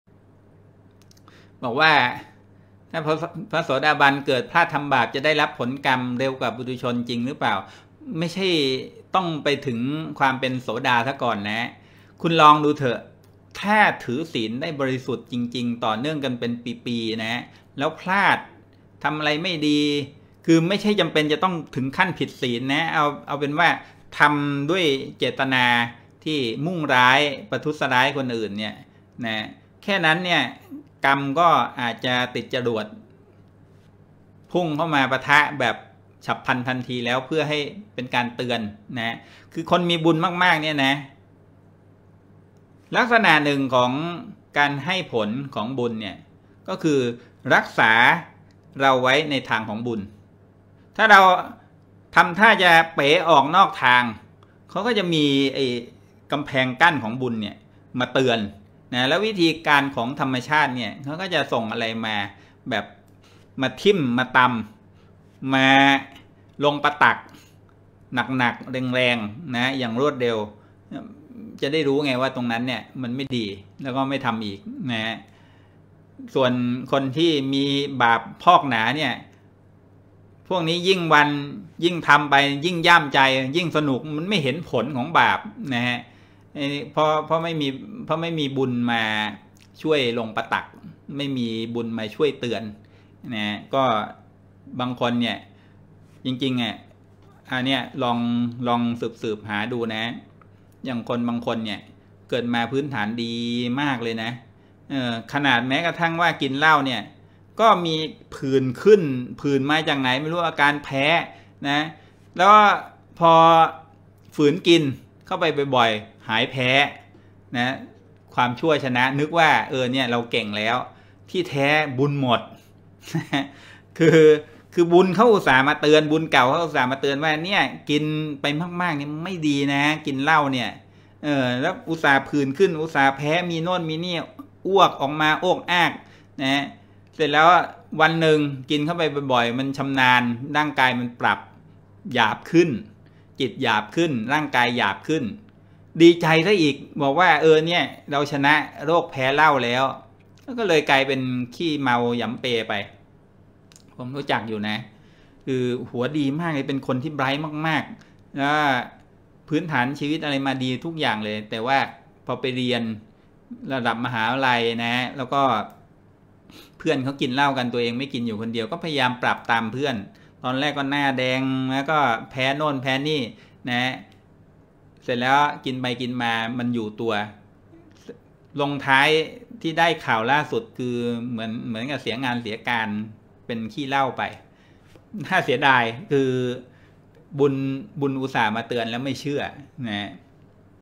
เคยได้ยินมาว่า ถ้าพระโสดาฯเกิดพลาดทำบาปจะได้รับผลของกรรมเร็วกว่าปุถุชนจริงป่าวครับ?
ปฏิบัติธรรมที่บ้าน ตอน ความต่างระหว่างพระโสดาบันกับพระอรหันต์
25 เมษายน 2563